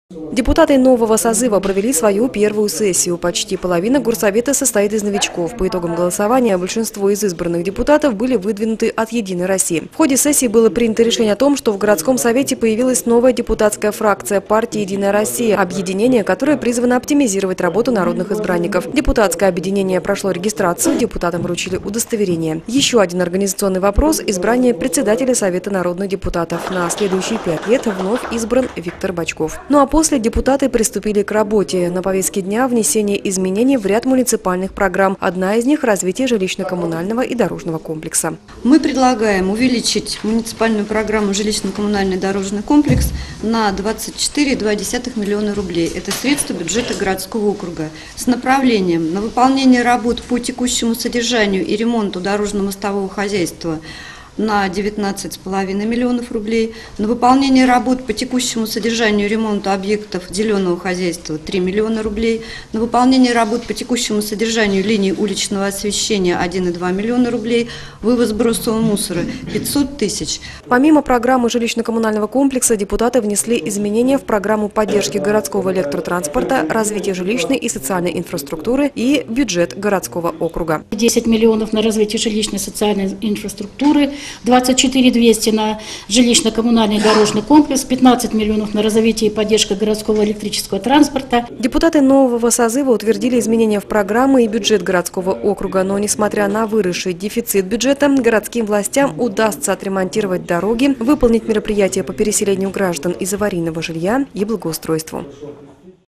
Первая сессия депутатов нового созыва
Совет народных депутатов Ленинск-Кузнецкого городского округа нового созыва провел свою первую сессию с участие главы города Вячеславом Телегиным. На сессии депутаты решили ряд организационных вопросов. Кроме того, на первом совместном заседании были внесены изменения в городские программы. В сюжете подробнее.